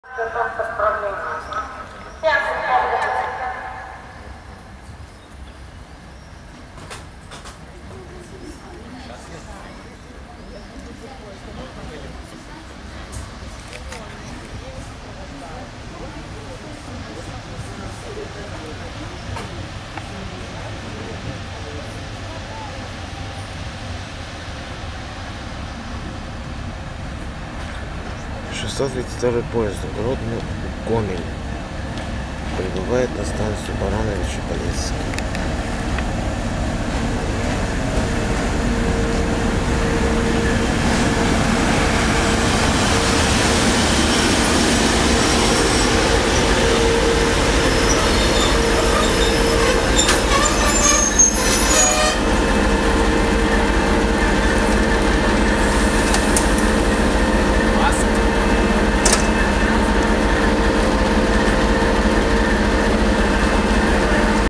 ТЭП70-0325 с поездом 632 Гродно - Гомель прибывает на станцию Барановичи-Полесские
Diesel locomotove TEP70-0325 with passenger train 632 arrive at Baranovichi-Polesskie station/Republic of Belarus. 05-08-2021.